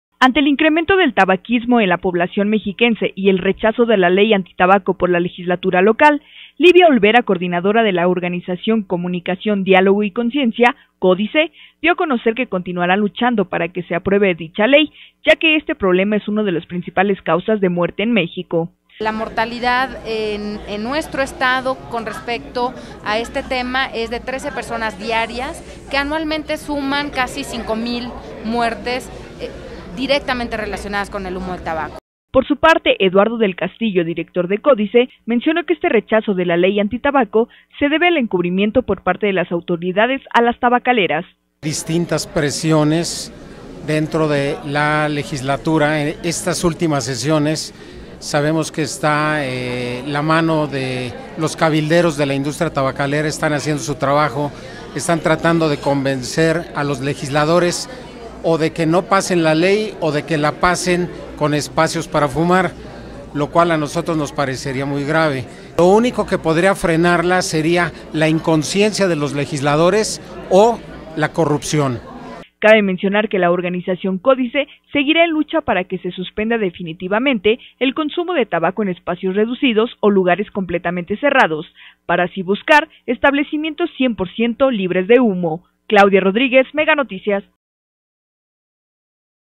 RECHAZAN LEGISLADORES LEY ANTITABACO
La legislatura local rechazó la ley antitabaco